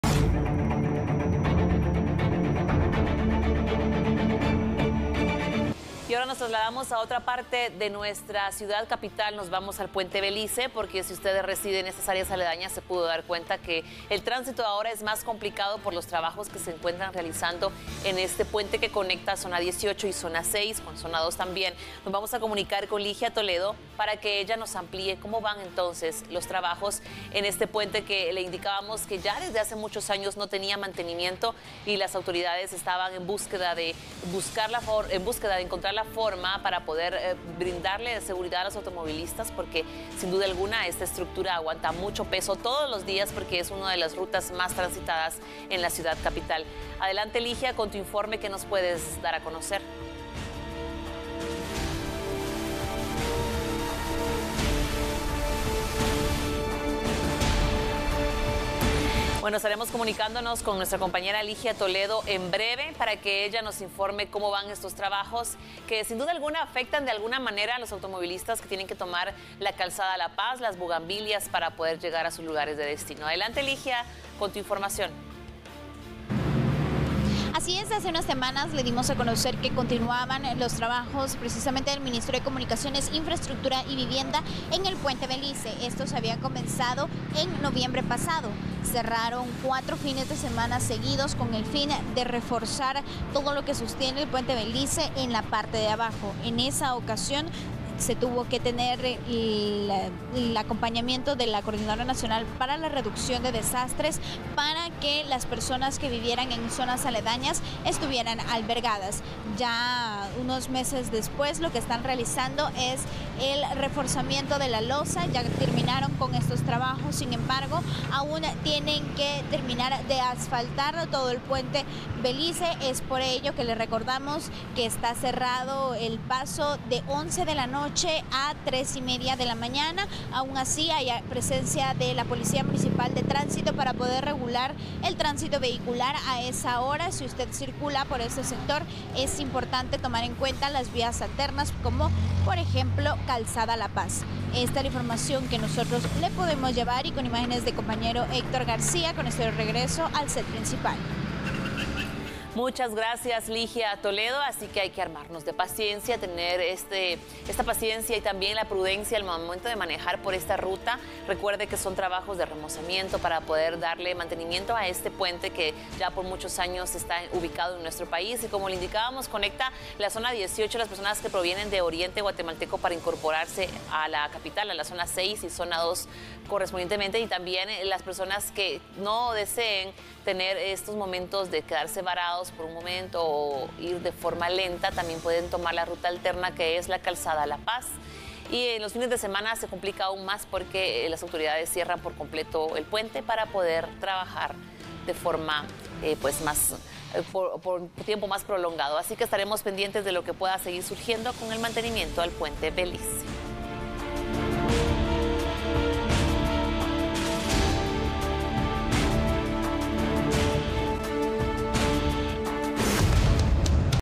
Siguen los trabajos en el Puente Belice
El MICIVI recordó horarios de los cierres parciales